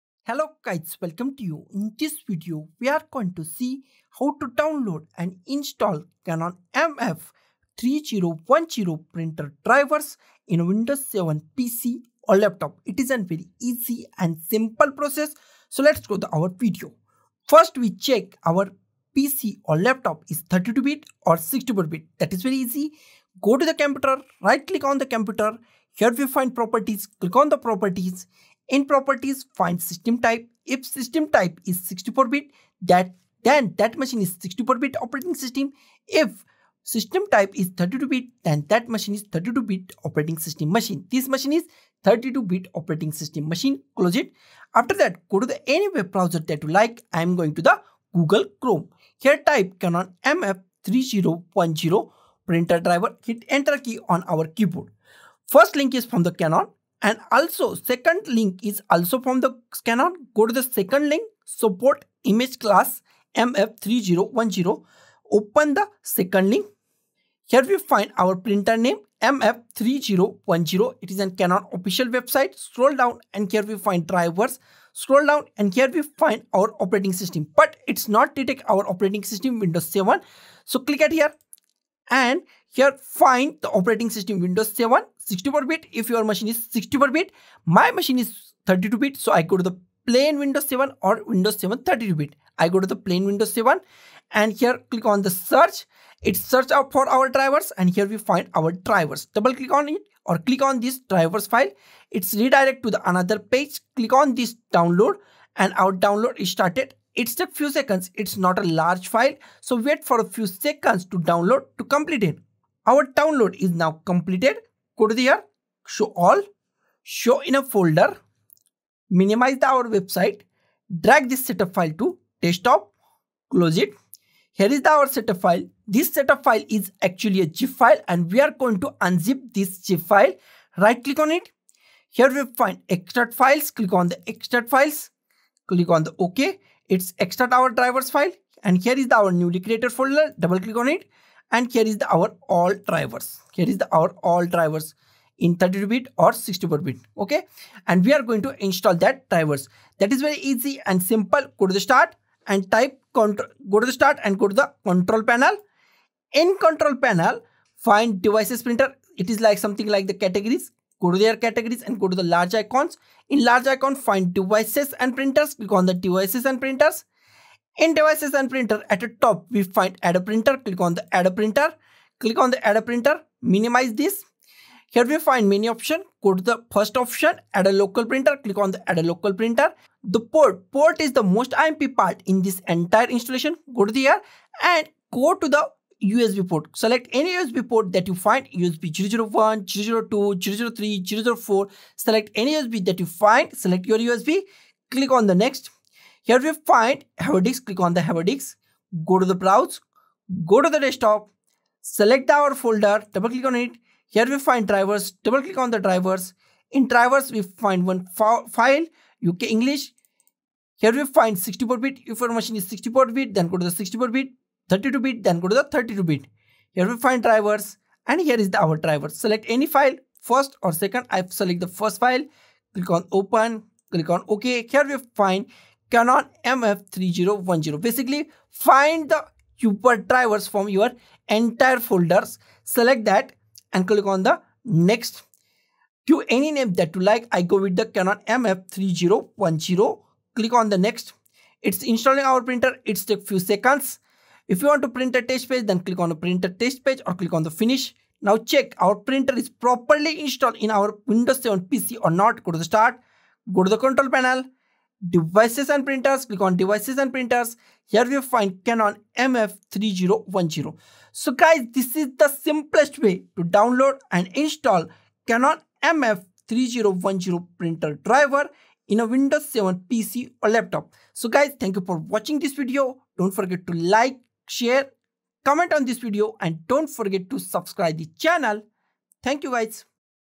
How to Download & Install Canon MF3010 Printer Drivers in Windows 7
🖨️ Struggling to set up your Canon MF3010 printer on Windows 7? We've got you covered! In this tutorial, we'll guide you through the process of downloading and installing the Canon MF3010 printer drivers on your Windows 7 PC using the official Canon support link.

🚀 Timestamps:
00:00 - Introduction
00:17 - check Windows 7 is 32 bit or 64 bit
00:44 - Downloading Canon mf3010 Printer Driver
02:04 - Extracting Downloaded Files
02:27 - Installing Canon mf3010 Printer Driver in windows 11
04:08 - check Canon mf3010 Printer is properly installed or not in Windows 11
04:20 - Conclusion

📌 Follow these simple steps:
1️⃣ Click on the official Canon link to access the MF3010 printer drivers: Canon Support Link
2️⃣ Select your operating system (Windows 7) and download the appropriate drivers.
3️⃣ Run the installer and follow the on-screen instructions.
4️⃣ Connect your Canon MF3010 printer to your Windows 7 PC when prompted.
5️⃣ Complete the installation process and start printing!

🛠️ Our step-by-step guide is designed to make the installation process hassle-free, even if you're not a tech expert. Ensure you choose the correct driver for your system from the official Canon link provided.

🚀 Get your Canon MF3010 up and running on Windows 7 with the ease of the official Canon support link. Let's dive into the installation process together.